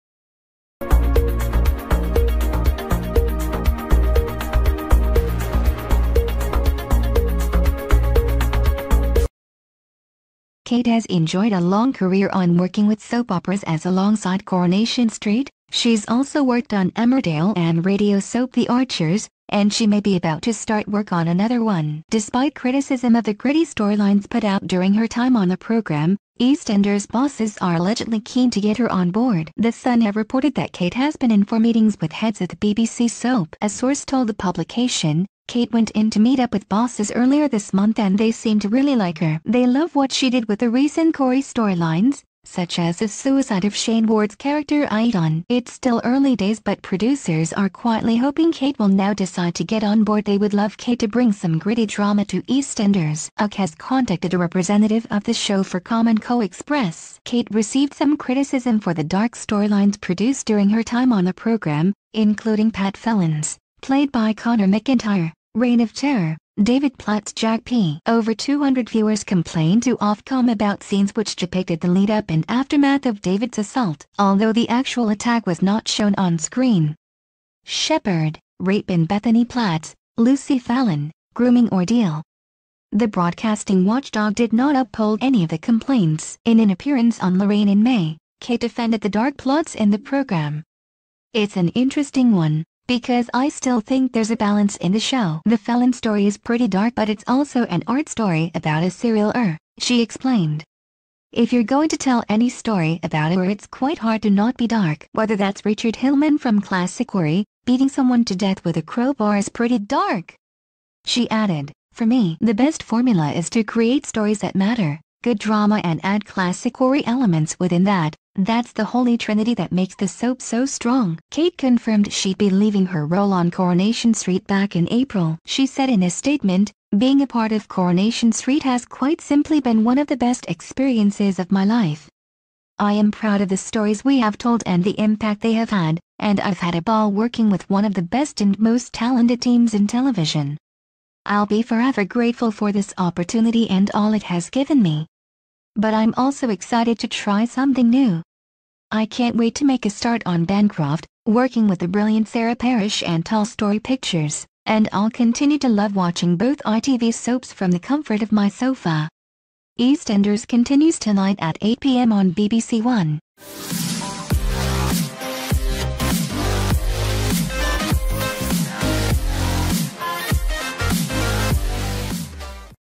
EastEnders news: Coronation Street producer Kate Oates in ‘meet-up’ with BBC soap bosses
Kate has enjoyed a long career on working with soap operas as alongside Coronation Street, she’s also worked on Emmerdale and radio soap The Archers, and she may be about to start work on another one.